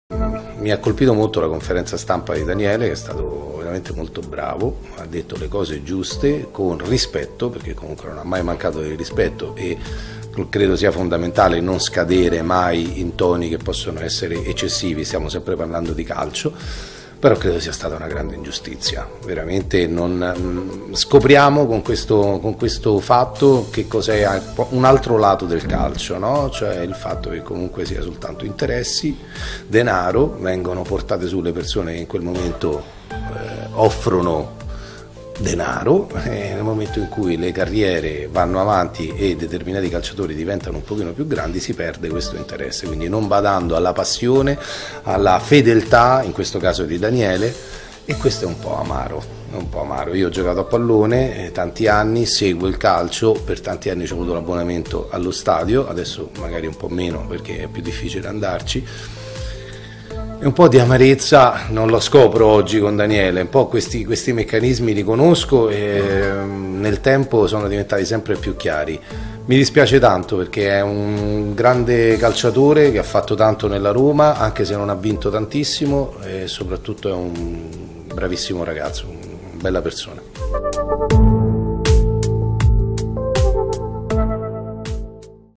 Daniele Liotti sull'addio di De Rossi alla Roma: 'E' una grande ingiustizia che lui non meritava'
In occasione della presentazione del film tv 'Duisburg - Linea di sangue', abbiamo incontrato Daniele Liotti che da ex calciatore e tifoso della Roma, ha commentato la vicenda De Rossi. "Sono senza parole - ha detto l'attore - credo sia stata una grande ingiustizia. Si scopre così un altro lato del calcio, quello in cui gli interessi e il denaro passano avanti a tutto, facendo dimenticare la passione e la fedeltà di un calciatore come De Rossi e questo è un po' amaro. Lui non meritava questo".

Serena Autieri e la passione per la Ducati: ‘La moto mi rende libera’